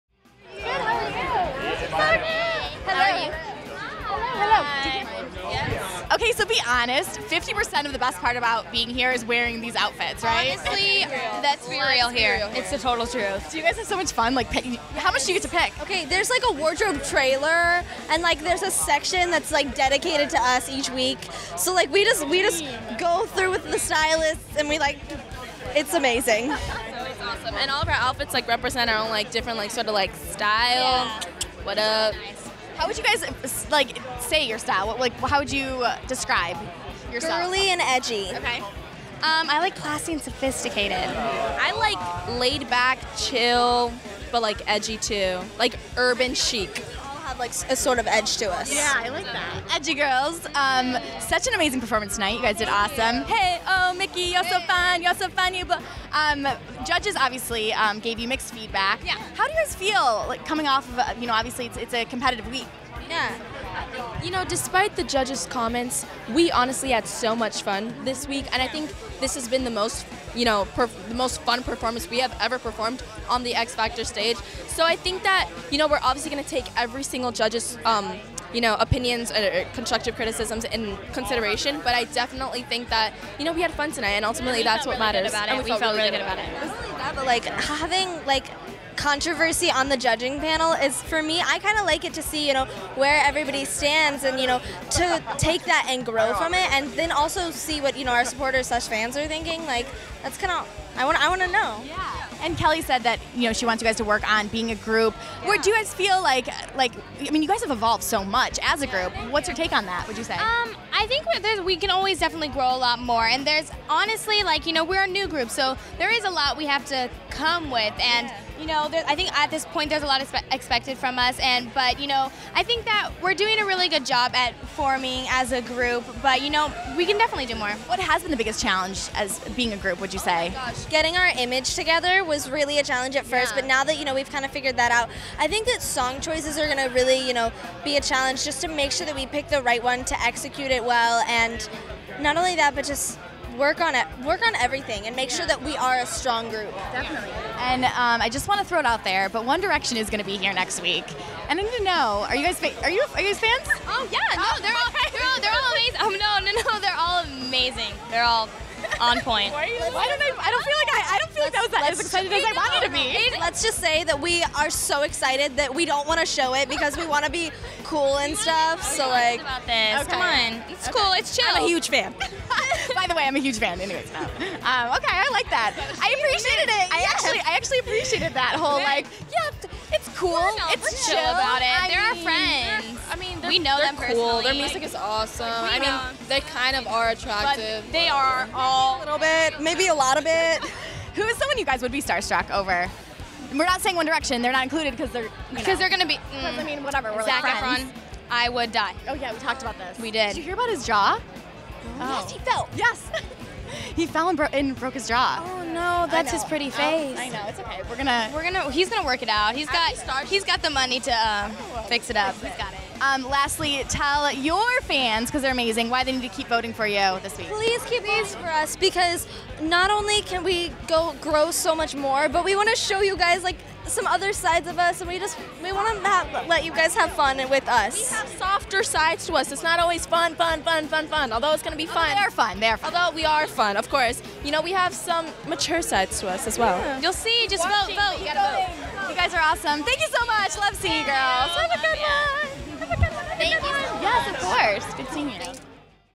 Sweet Suspense Fangirl Over One Direction!
We caught up with the girls of Sweet Suspense last night!

The lovely ladies talked about this week's performance and tough comments from the judges, as well as how they're working on evolving as a group.

Sweet Suspense also revealed their fangirl excitement for One Direction's performance next week! Thanks ladies!

For any goodies, send to:
Hollywire
9701 Wilshire Blvd # 8,